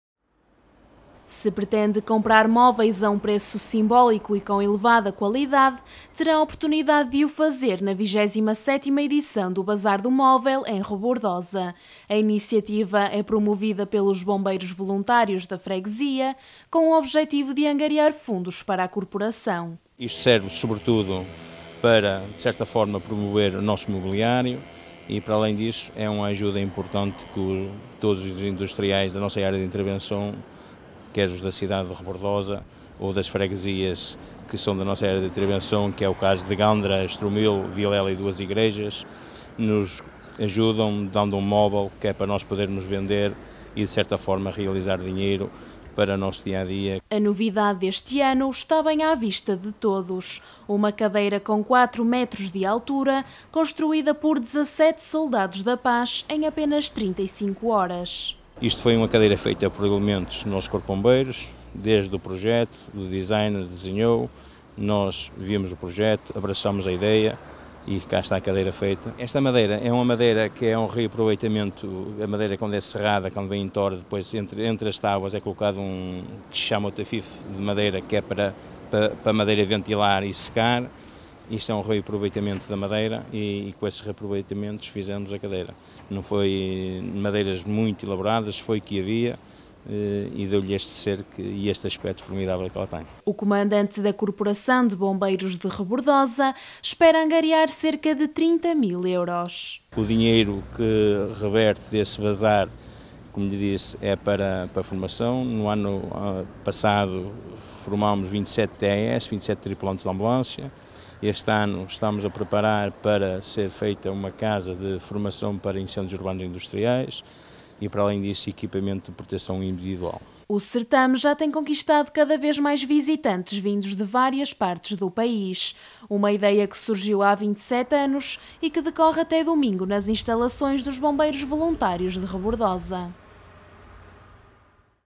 Empresas de mobiliário do concelho de Paredes apoiam Bazar do Móvel
Se pretende comprar móveis a um preço simbólico e com elevada qualidade terá a oportunidade de o fazer na 27ª edição do Bazar do Móvel, em Rebordosa. A iniciativa é promovida pelos Bombeiros Voluntários da freguesia com o objetivo de angariar fundos para a corporação.